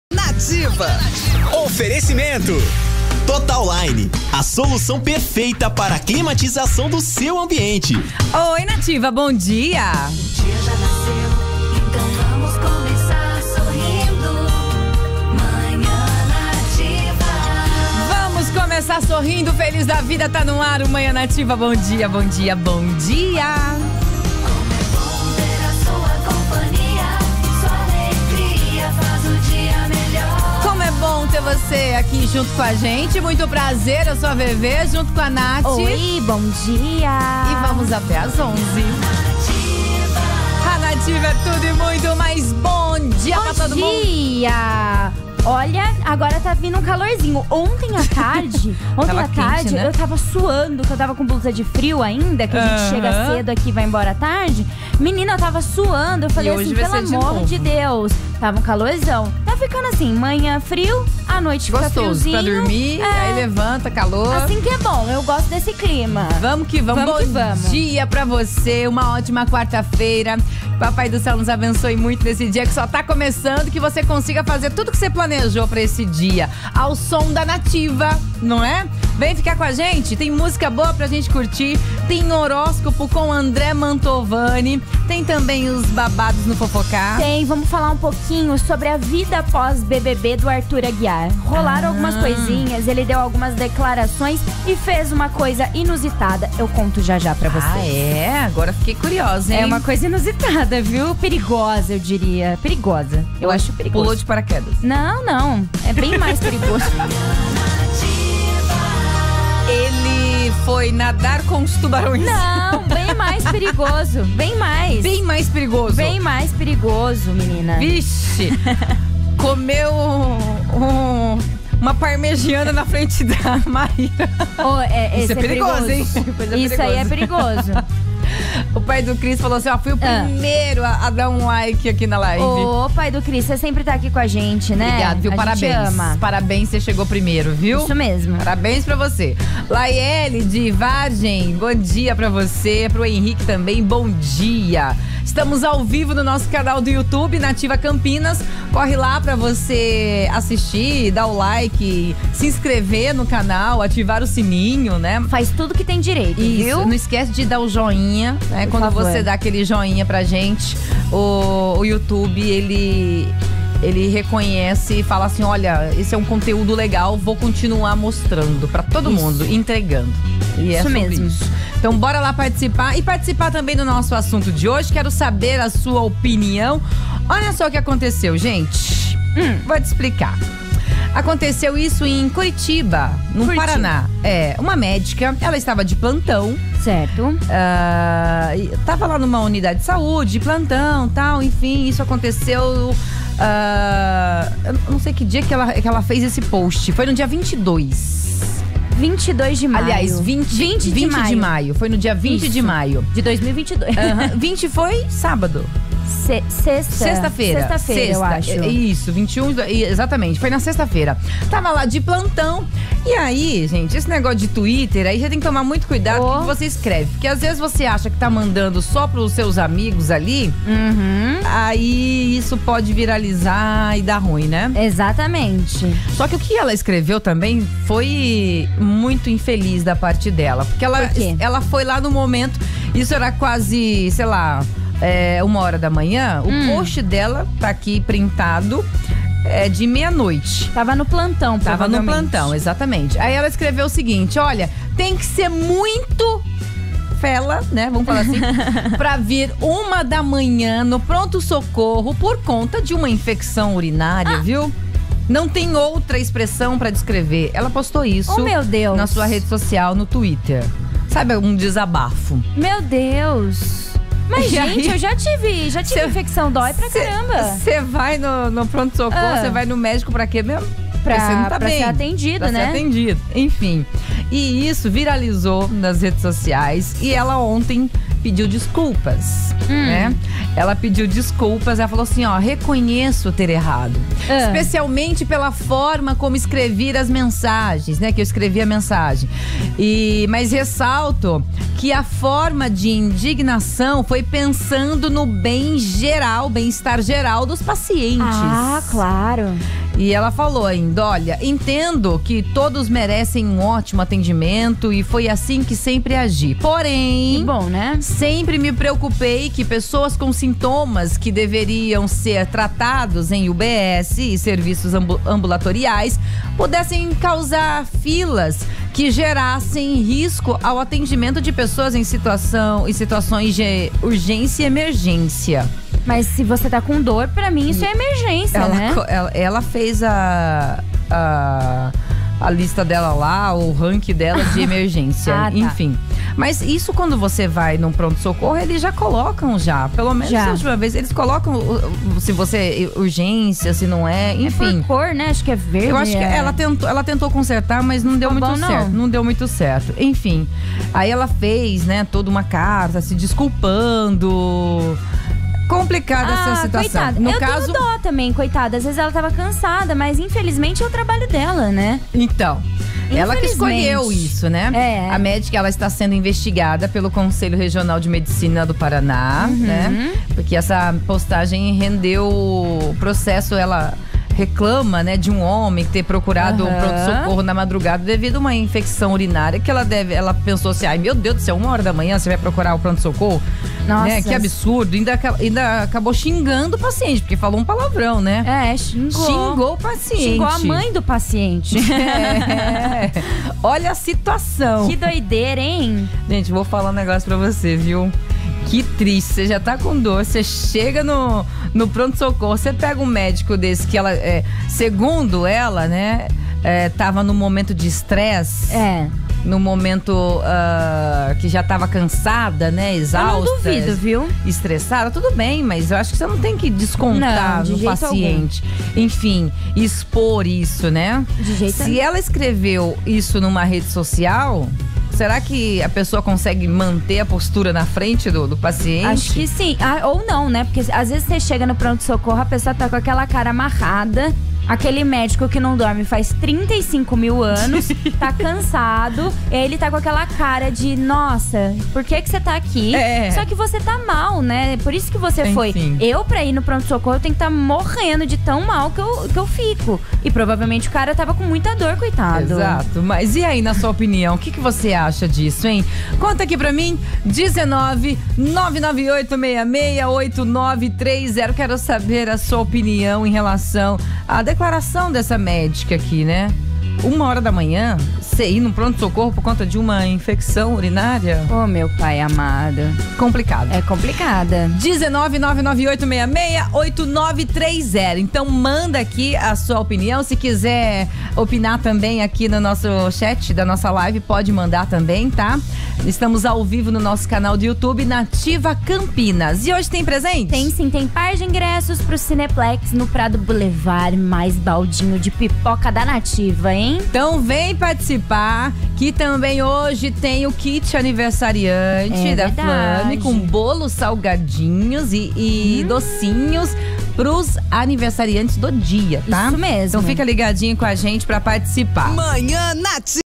“Momento de estresse”, diz médica que xingou paciente no Twitter
A médica da cidade de Almirante Tamandaré, na Região Metropolitana de Curitiba (PR), que usou as redes sociais para xingar um paciente durante seu plantão na unidade de saúde, pediu desculpas nesta terça-feira (24/5).

“Reconheço ter errado, especialmente pela forma como escrevi as mensagens, mas ressalto que a forma de indignação foi pensando no bem-estar geral dos pacientes”, escreveu a médica em nota ao G1.

“Entendo que todos mereçam ótimo tratamento e foi assim que sempre agi. Porém ,sempre me preocupei que pessoas com sintomas que deveriam ser tratados em UBS e serviços ambulatoriais pudessem causar filas que gerassem risco ao atendimento de pessoas em situações de urgência/emergência”, justificou.

A médica está sendo investigada pelo Conselho Regional de Medicina do Paraná (CRM-PR). Na postagem que rendeu o processo, ela reclama de um homem por ter procurado o pronto-socorro de madrugada devido a uma infecção urinária.

A ação do órgão de medicina veio à tona nessa segunda-feira (23/5), dois dias após a postagem, que viralizou no Twitter.

Segundo o cadastro da profissional no CRM, ela mora em Curitiba e está inscrita na autarquia de Almirante Tamandaré desde setembro de 2021. A reportagem da Banda B descobriu que a médica atua em uma Unidade de Pronto Atendimento (UPA) do município metropolitano, como médica plantonista.